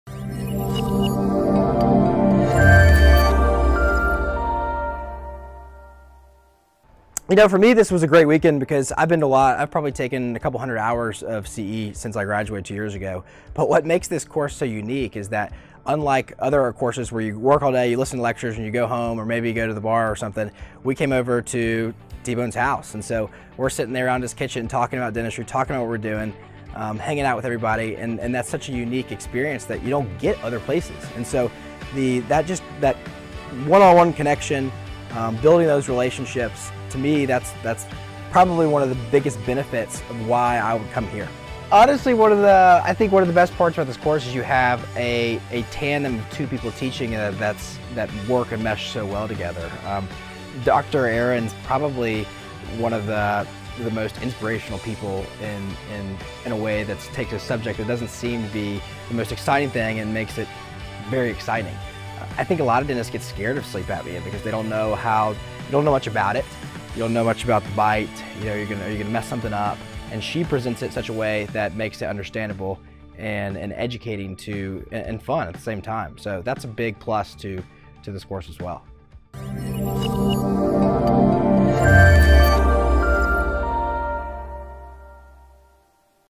Sleep 101 Testimonial with Dr. Sully Sullivan (90 sec)
Dr. Sully Sullivan shares a quick thought on the Sleep 101 seminar from 3D Dentists.